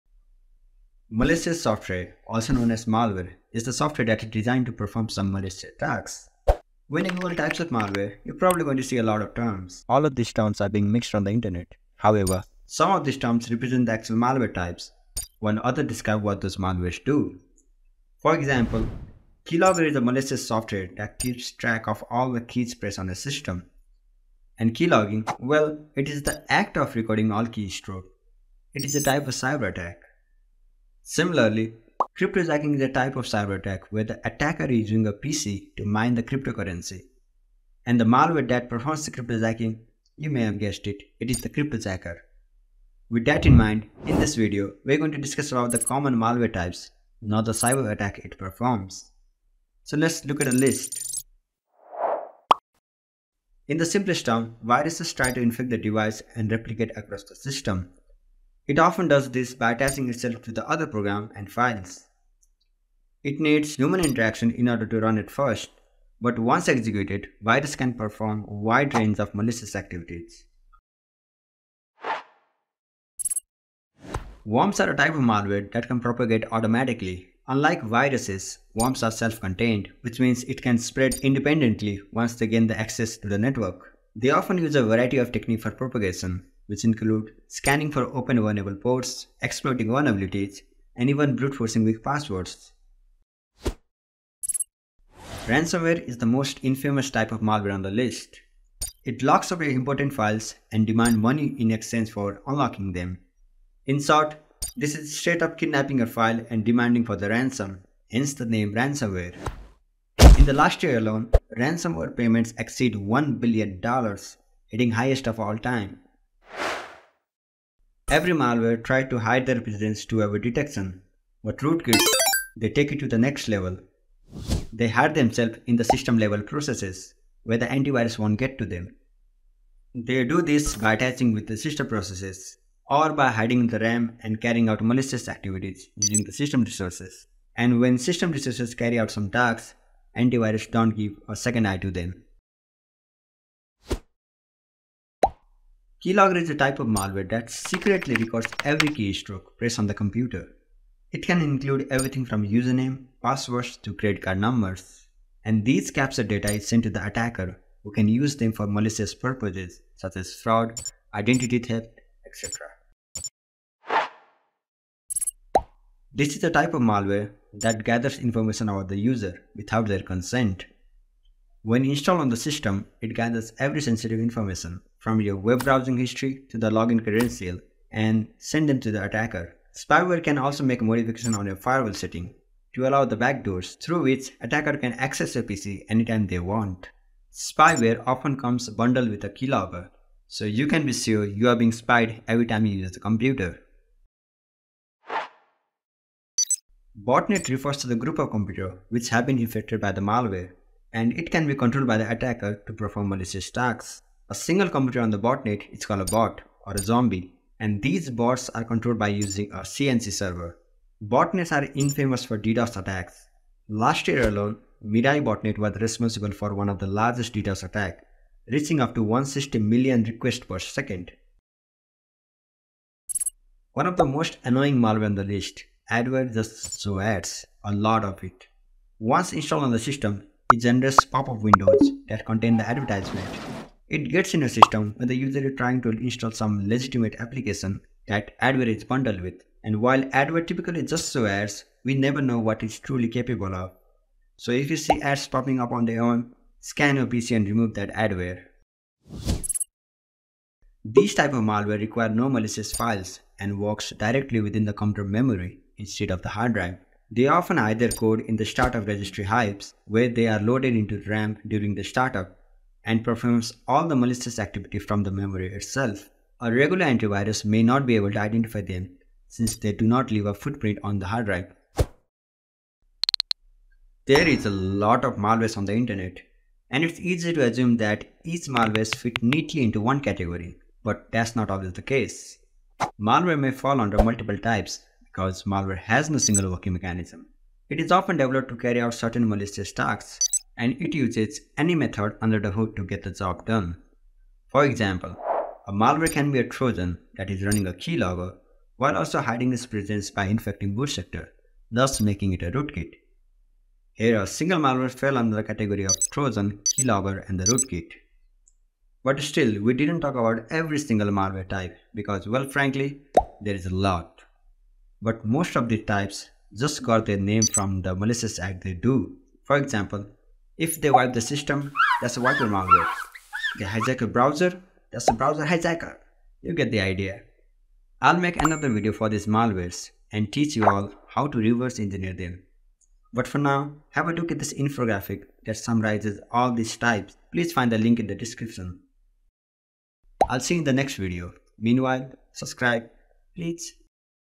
Types of Malwares & Attacks Explained - With Links | CompTIA Sec+
There are a lot of malwares on the Internet. This video covers about the most common malware types in brief.

It's a shame I forgot to mention about Trojan. Sorry about that.

Trojan disguise themselves as a legitimate application and trick user to install them. Once installed on the system, it can do any bad things you can imagine.

Timestamps:
0:00 - Introduction
0:50 - Malware Explanation
4:34 - Further Types of Malwares
5:28 - Final Words

Malware analysis is a great way to understand how these malwares function in real life. I actually did my graduation thesis on the Malware analysis where I demonstrated the static and dynamic analysis of the malware.

If you want to perform analysis yourself, setup the isolated environment. You can get your hands on the malware by following these steps:

1. Go to Google.
2. Type "Malware Sample GitHub Repository"
3. Clone the repo.

Note: Malwares are no joke. The analysis should be performed in a contained environment. You could infect your device if not handled properly.

With that said, Keep learning. If you learned something new, you'll definitely want to get connected with us.